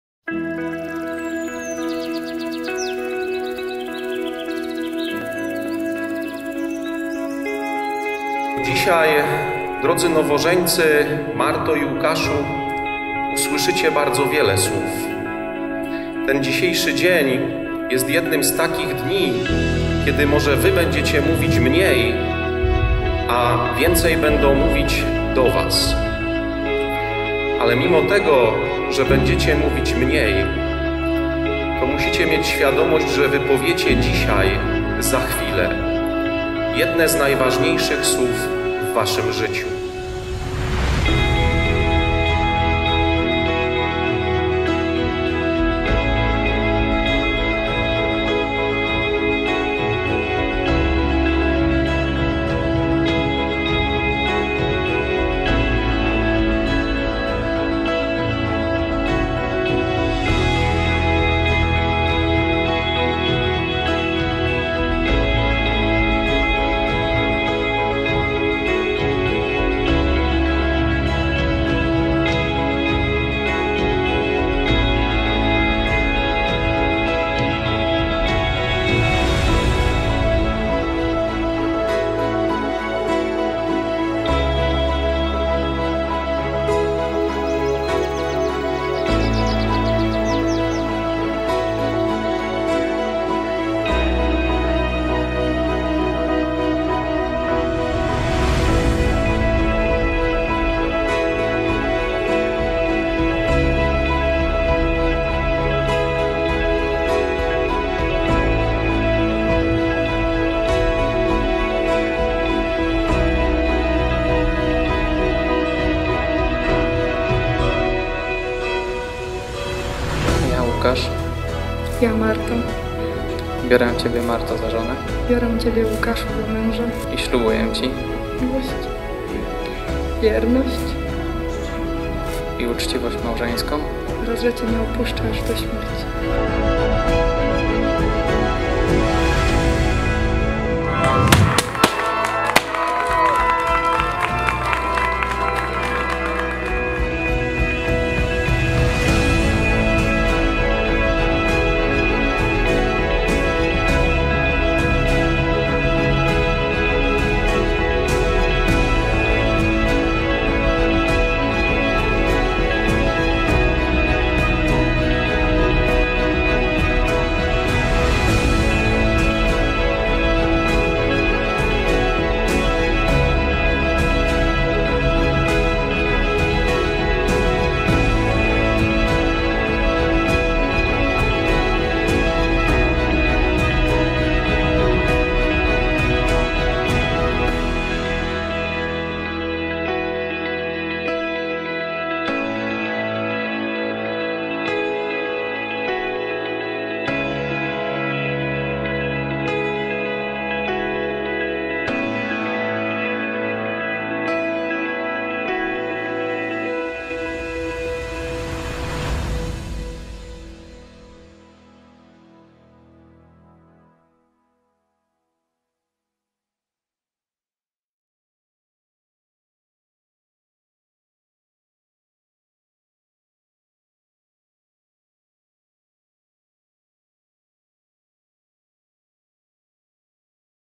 Teledysk Marta i Łukasz - Love Wedding Story - jakmarzenie.pl
Teledysk ślubny Marty i Łukasza
Realizacja: jakmarzenie.pl